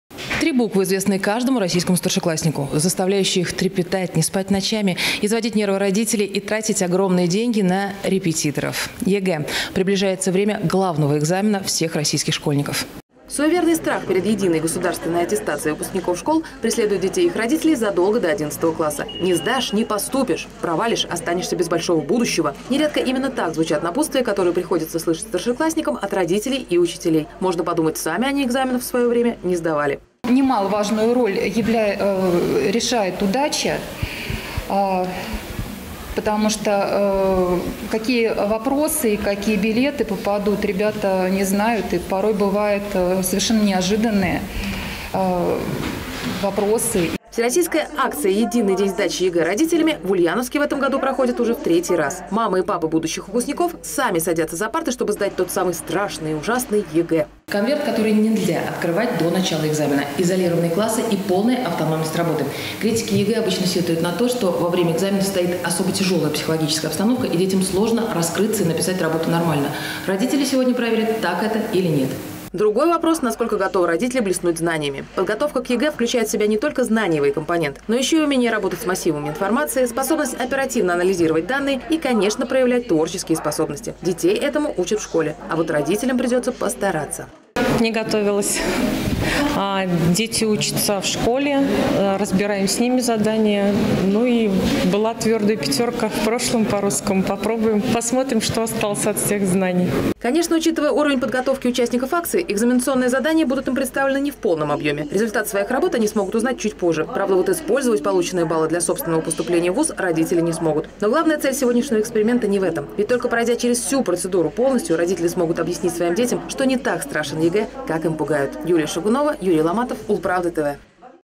Пошли на три буквы. Родители ульяновских школьников сдали ЕГЭ
Три буквы, не покидающие мысли каждого российского старшеклассника и заставляющие их трепетать и не спать ночами. Изводить нервы родителей и тратить кругленькие суммы на репетиторов. ЕГЭ. Приближается время главного экзамена для миллионов российских школьников.